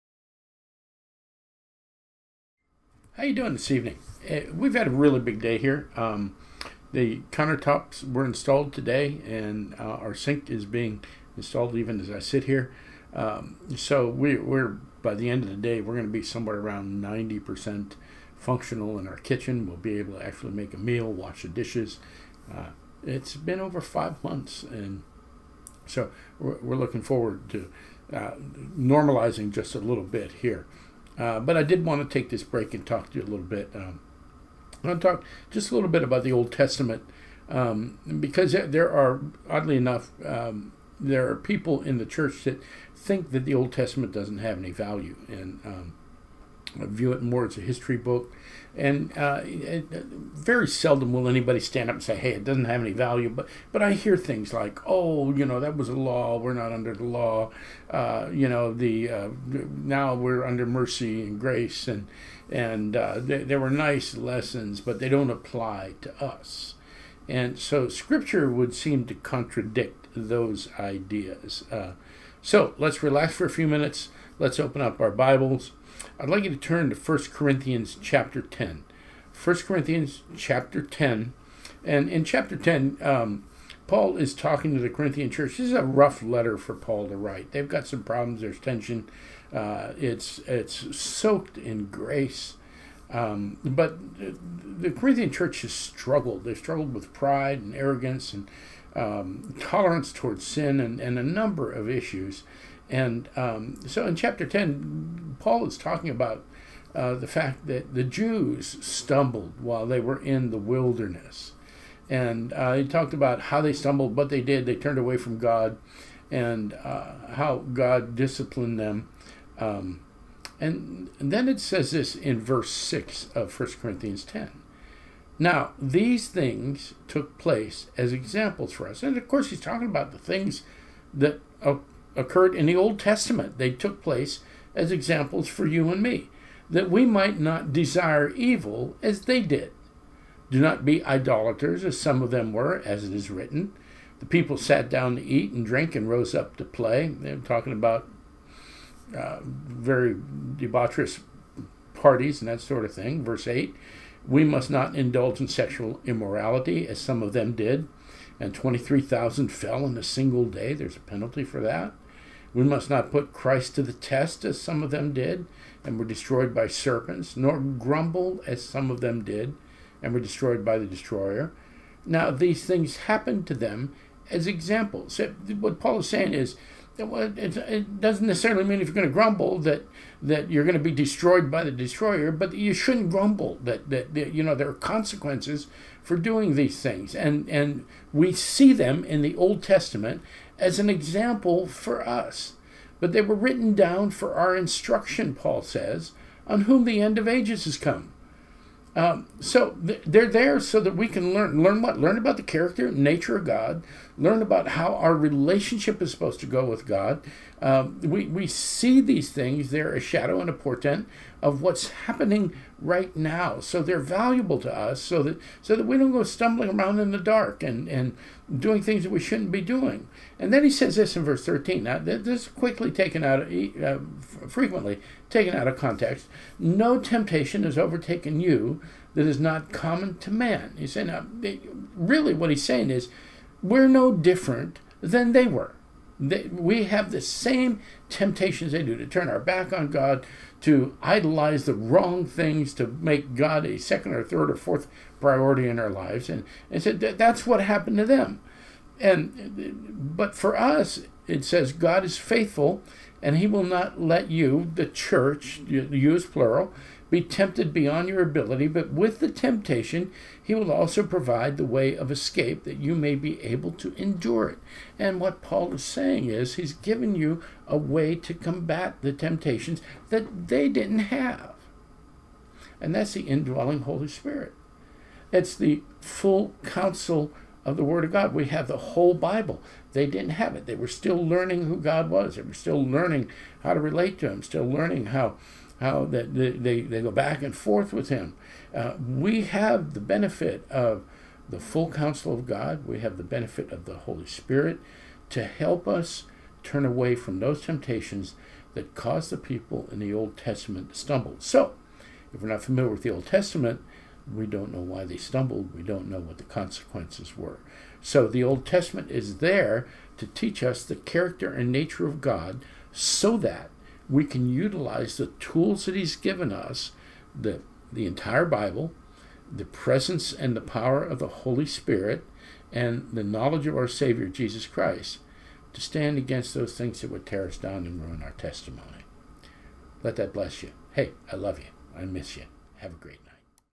Does the Old Testament have any value to a believer?
Is it just a history book? Has it been nullified by grace?